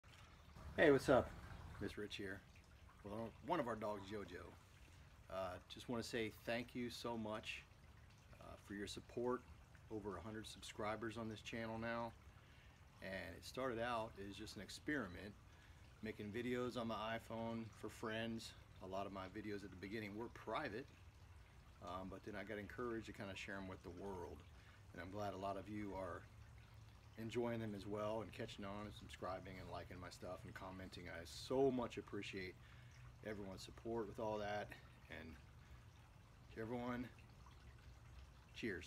100+ SUBSCRIBERS!! Thank you!!
Hey what's up?! Here in the backyard with one of our dogs, Jojo, beautiful day in the Pacific Northwest, USA.
I just want to say thank you for your support for this channel. It started out as an experiment, making drum covers for friends on the iPhone and at the beginning my videos were mostly private. Then I got encouraged to share them with the world. This also led to me getting back into playing in bands.
To all of you, Cheers!

Rich West